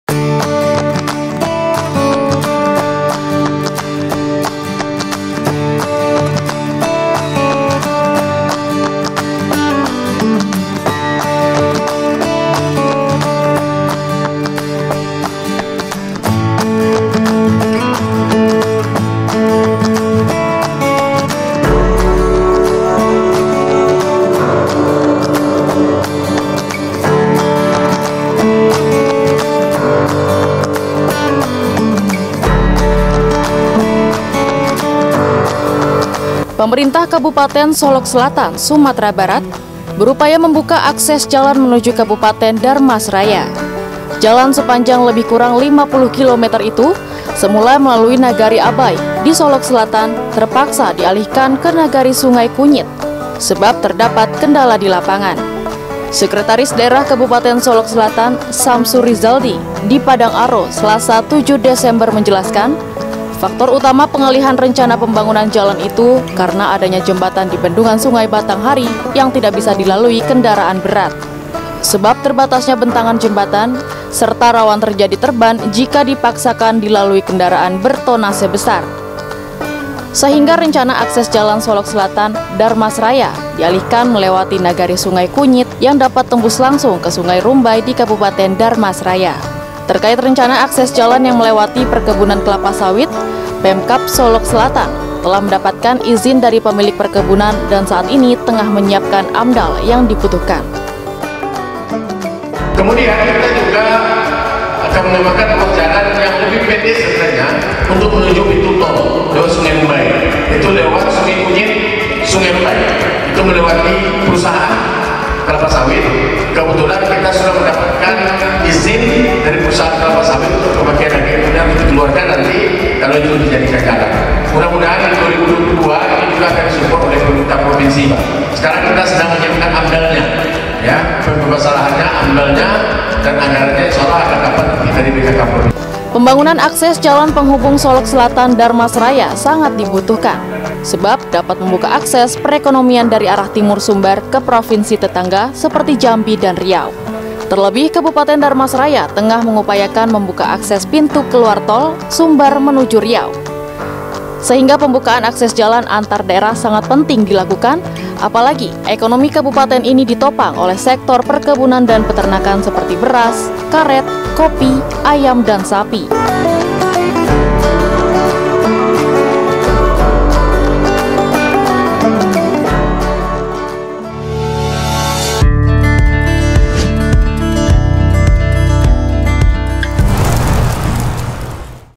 Gara-gara Akses Tol, Kab. Solok Selatan Ingin Ikutan Maju, Segera Bangun Jalan ke Kab. Dharmasraya
Upaya pembukaan akses jalan menuju Kabupaten Dharmasraya dari Kabupaten Solok Selatan sepanjang 50 kilometer terus dilakukan. Jalan penghubung yang awalnya melalui Nagari Abai di Solok Selatan, harus dialihkan ke Nagari Sungai Kunyit karena terdapat beberapa kendala di lapangan. Pemkab Solok Selatan berharap dengan terbukanya akses jalan ke Dharmasraya, membuat daerahnya bisa dengan cepat menuju pintu tol Dharmasraya yang direncanakan akan terhubung dengan jalan tol Rengat-Pekanbaru.

Info selanjutnya pada ulasan video berikut!